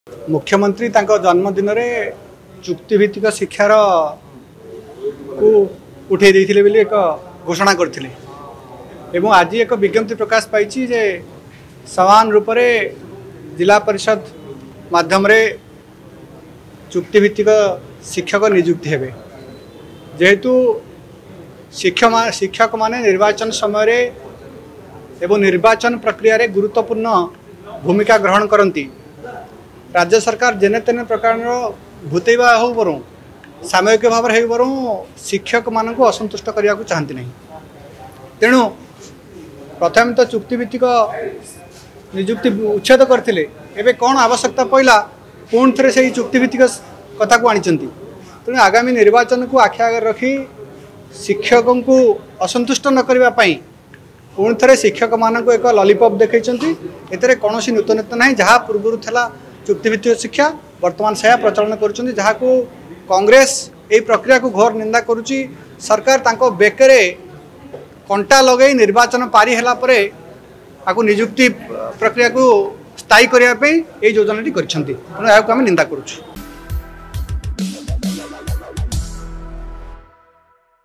ଶିିକ୍ଷକଙ୍କୁ ଲଲିପପ ଦେଖାଇଛନ୍ତି ସରକାର
ସରକାର ଶିକ୍ଷକଙ୍କ ବେକରେ କଣ୍ଟା ଲଗାଇ ନିର୍ବାଚନ ପାରି ହେଲା ପରେ ନିଯୁକ୍ତ ପ୍ରକ୍ରିୟାକୁ ସ୍ଥାୟୀ କରିବାକୁ ଯୋଜନା କରିଛନ୍ତି